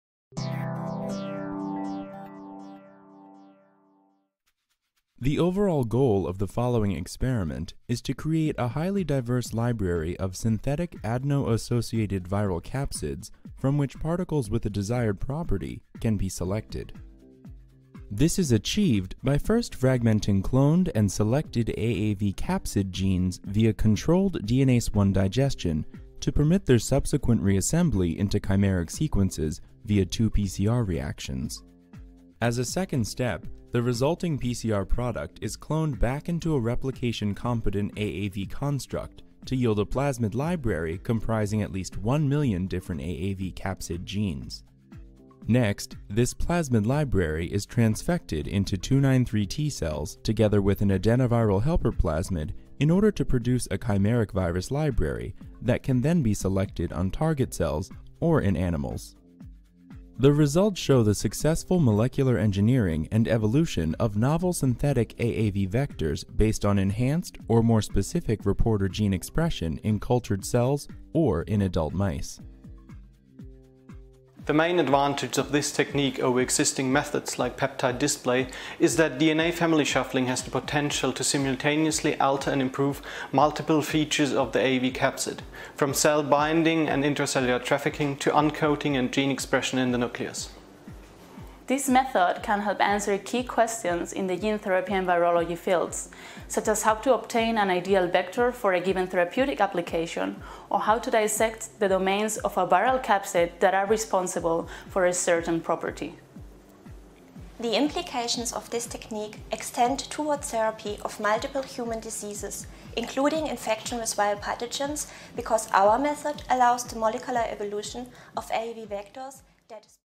Engineering & Evolution Of AAV Gene Therapy Vectors Via DNA Family Shuffling l Protocol Preview
Engineering and Evolution of Synthetic Adeno-Associated Virus (AAV) Gene Therapy Vectors via DNA Family Shuffling -  a 2 minute Preview of the Experimental Protocol

Eike Kienle, Elena Senís, Kathleen Börner, Dominik Niopek, Ellen Wiedtke, Stefanie Grosse, Dirk Grimm
Heidelberg University, Cluster of Excellence CellNetworks, Department of Infectious Diseases, Virology; Heidelberg University, Department of Infectious Diseases, Virology;

We demonstrate the basic technique to molecularly engineer and evolve synthetic Adeno-associated viral (AAV) gene therapy vectors via DNA family shuffling. Moreover, we provide general guidelines and representative examples for selection and analysis of individual chimeric capsids with enhanced properties on target cells in culture or in mice.